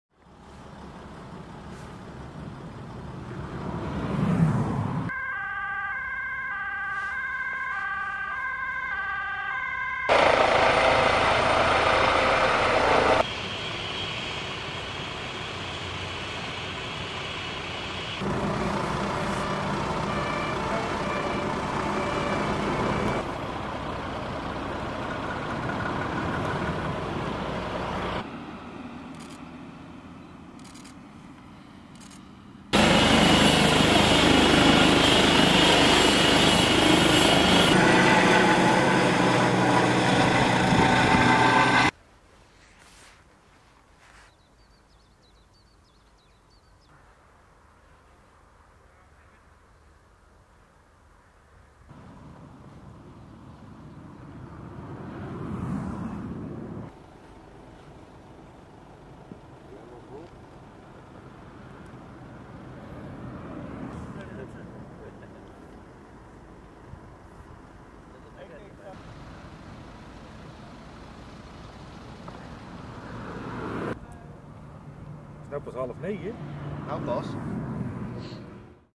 Ongeval A59
Bij een ongeval op de A59 ter hoogte van Oosterhout zijn zondagmorgen rond 7.30 uur zeker twee gewonden gevallen.

Mathijs Bertens/ Stuve fotografie